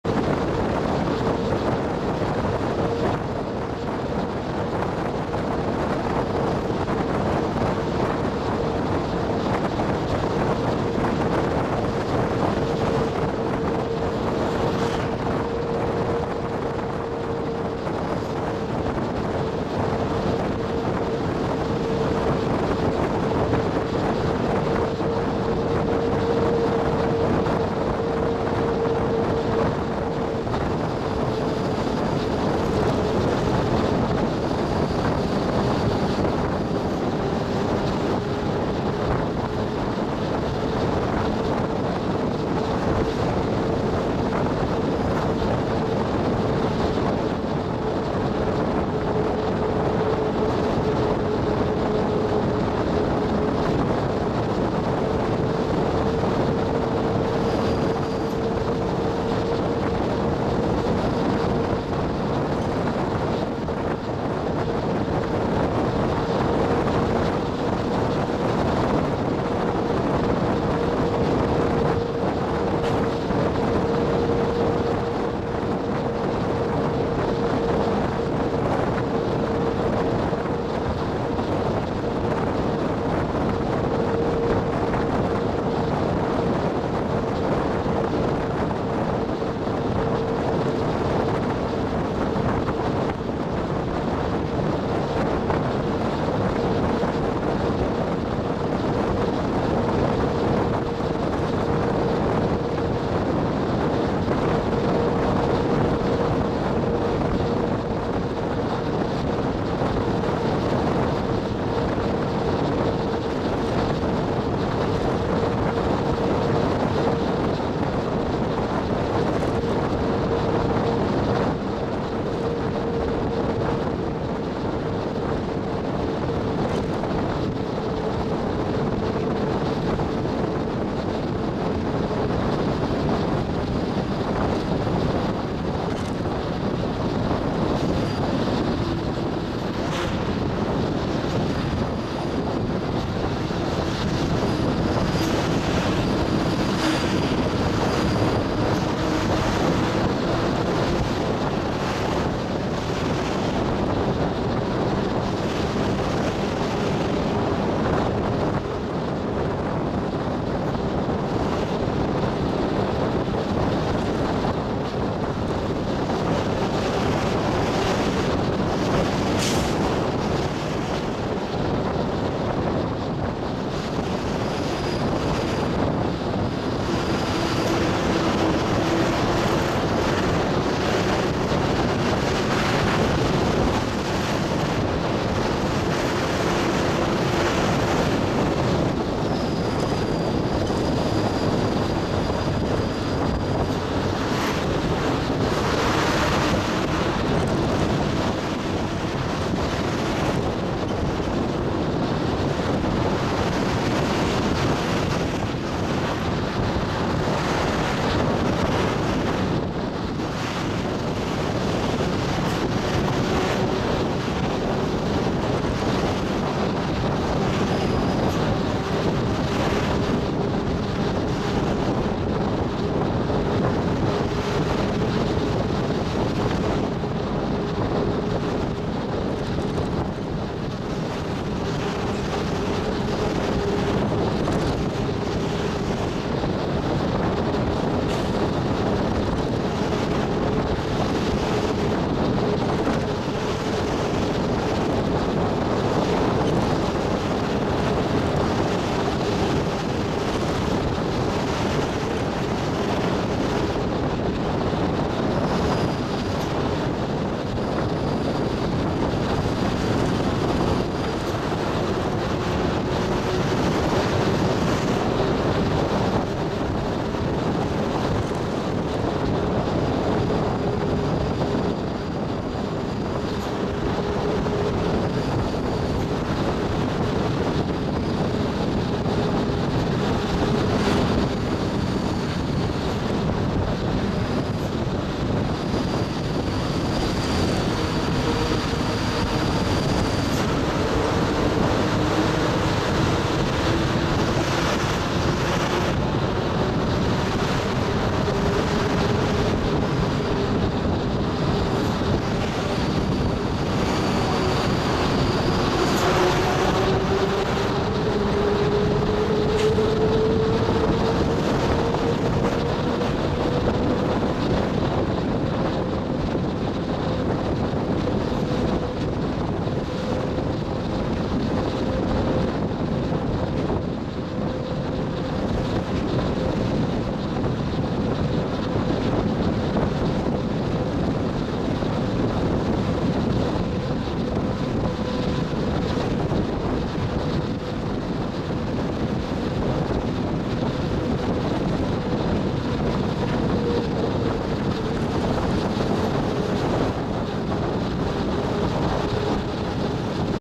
Bus window view at HINOEMATA village.
Hinoemata village is the lowest population density village in Japan.
And one of the most distant area  from Tokyo by time.
Nowadays relativity  easy to get here than ever , but still secluded regions.